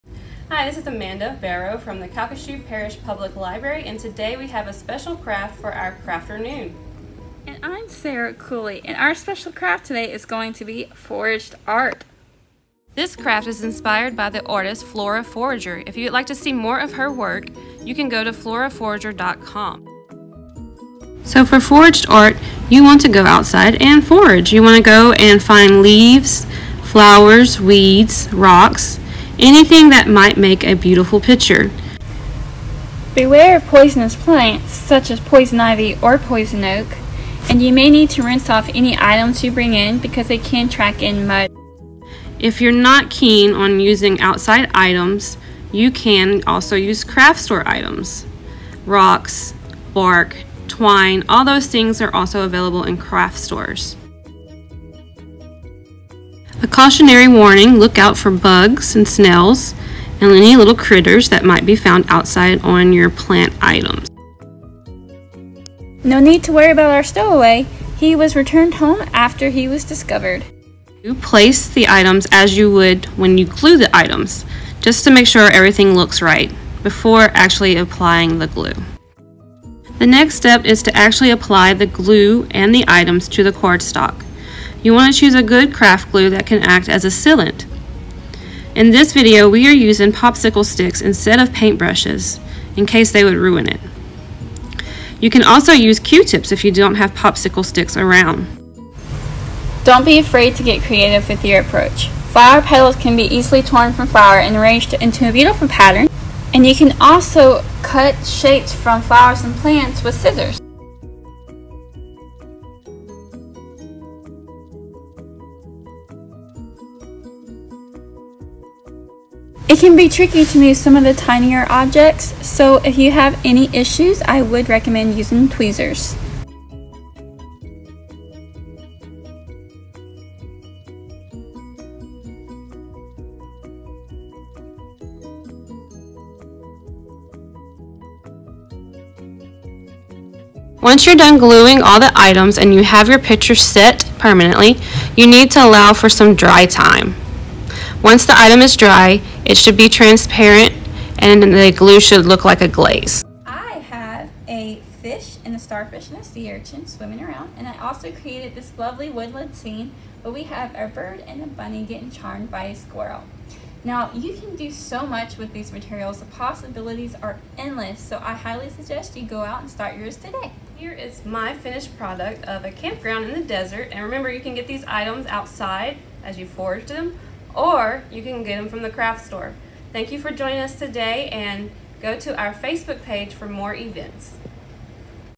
Crafternoon for Adults: Foraged Art
Get ready for the holidays Ms. Amanda and Ms. Sara. They're here to teach you how to forage and create art from your own yard! An inexpensive gift that is sure to bring joy this holiday season.